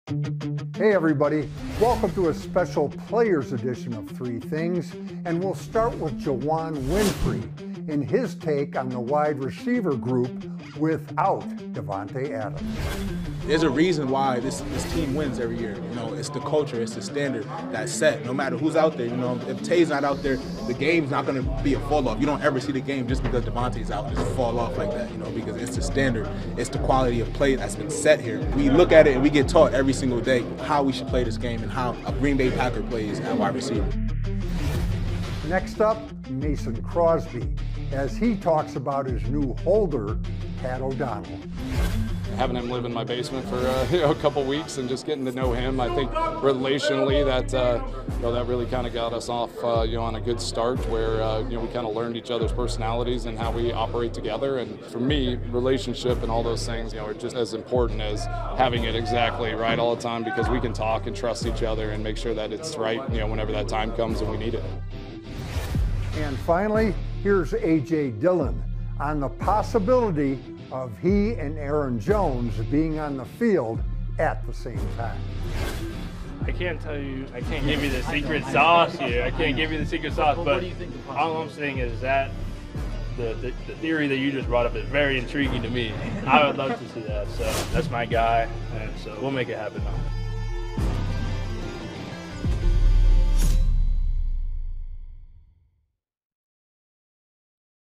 Three Things – Players’ Edition | Week 2 OTAs
In this special “players” edition of Three Things, Juwann Winfree, Mason Crosby and AJ Dillon talk about the wide receivers room, a new holder and double-trouble in the Green Bay Packers' backfield.